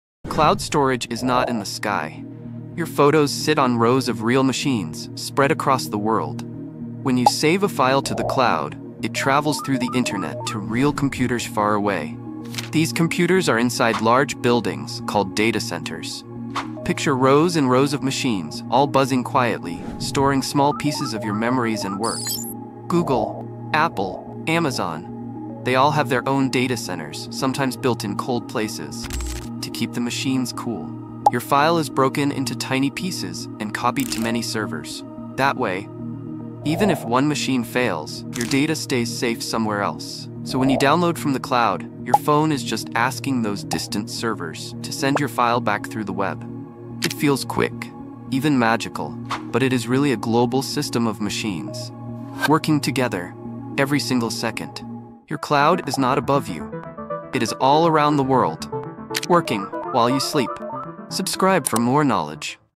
Where Does Cloud Storage Actually Live? ☁️
Wait… My Files Are Stored Where?! 🤯
This Is What the Cloud Really Looks Like 😳

Where do your photos and files really go when you upload them to “the cloud”?
They’re not floating above us — they’re sitting in massive, secret data centers around the world.

In this short, I break down how cloud storage actually works, from your phone to global server farms run by Google, Apple, and Amazon.
If you’ve ever wondered where your digital life truly lives, this one will surprise you.